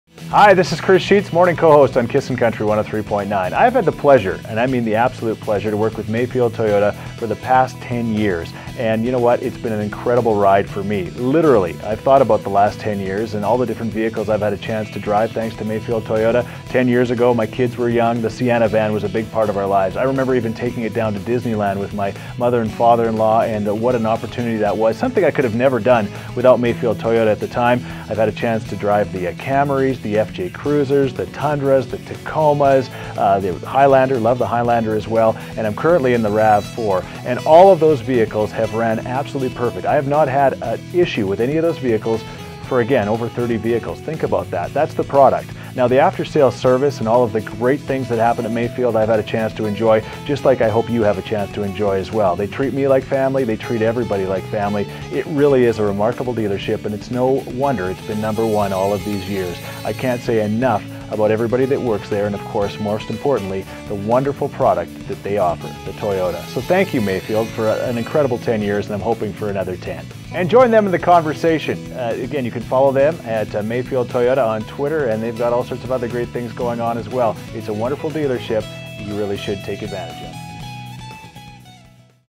Testimonial: Chris Scheetz, CISN Country 103.9 Radio Host | Edmonton | Toyota Rav 4
Let CISN Country 103.9 FM Radio Host tell you why he has been getting his vehicles from us for 10 years! He talks about the models he has driven from Tundra all the way to Rav 4 and everything in between. Have a peek! We are looking forward to providing our past, present and future customers with the same quality service!